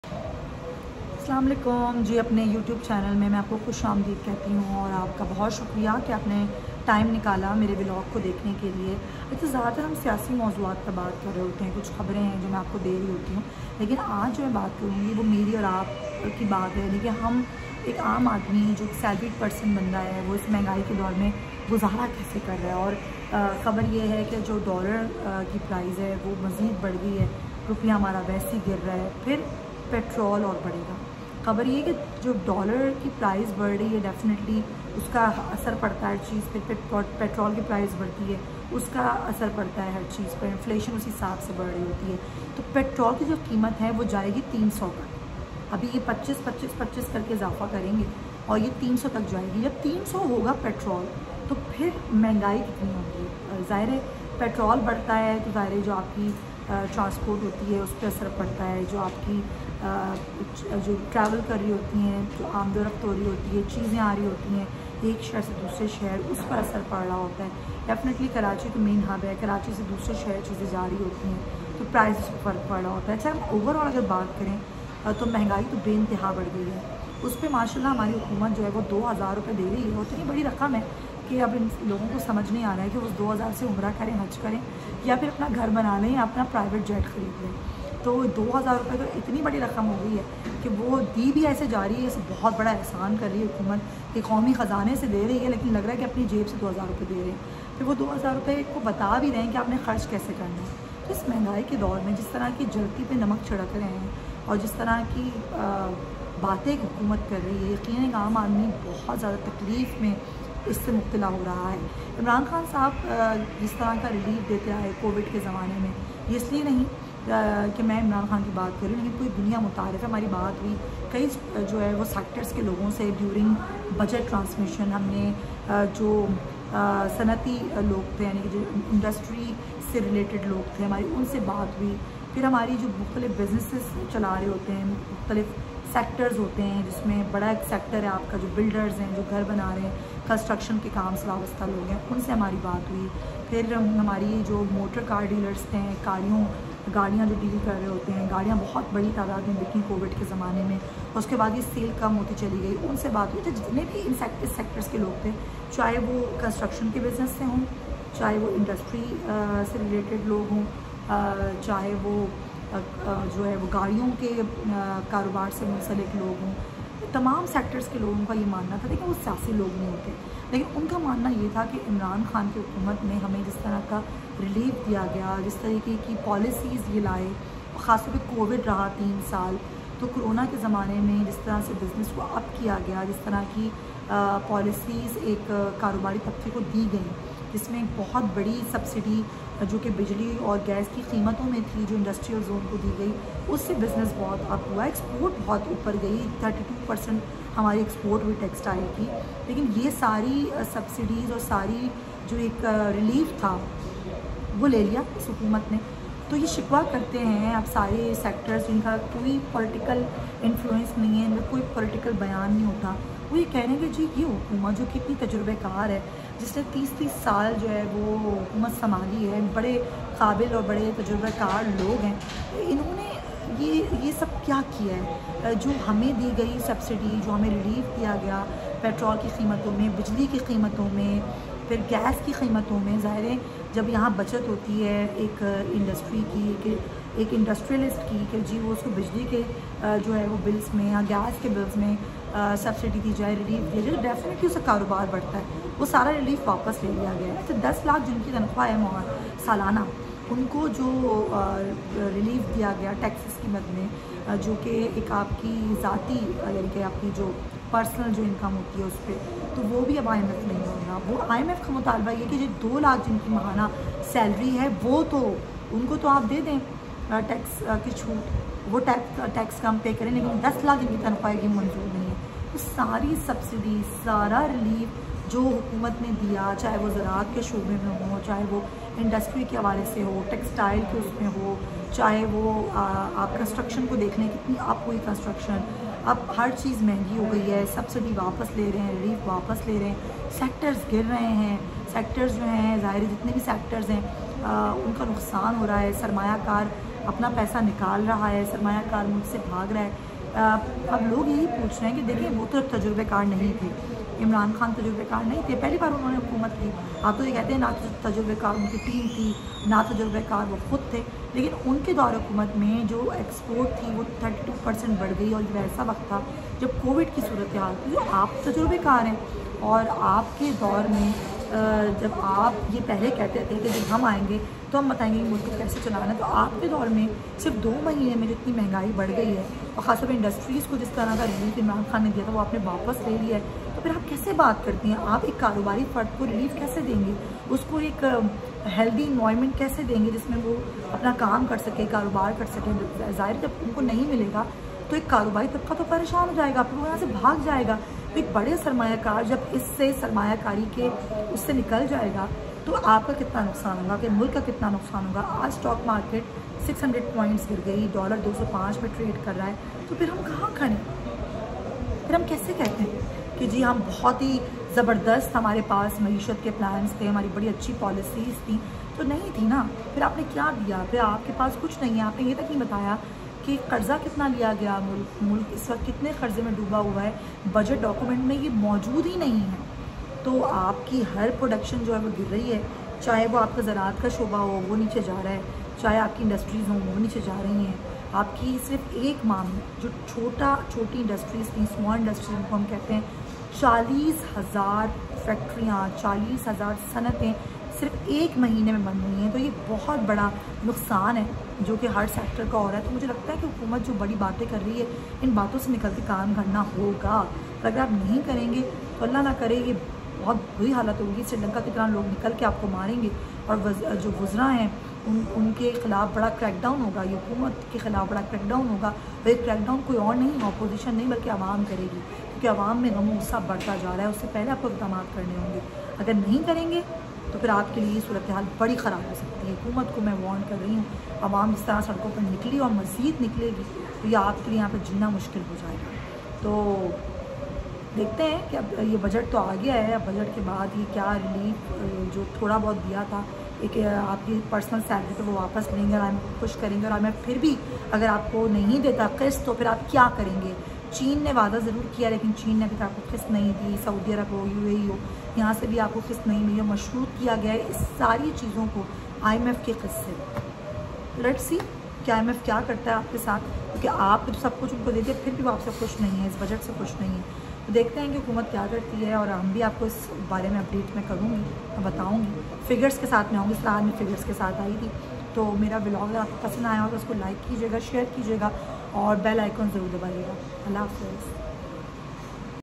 Govt failed to provide relief in budget // IMF // budget // inflation
Govtofpakistan budget 2022 increased inflation and imf reject this budget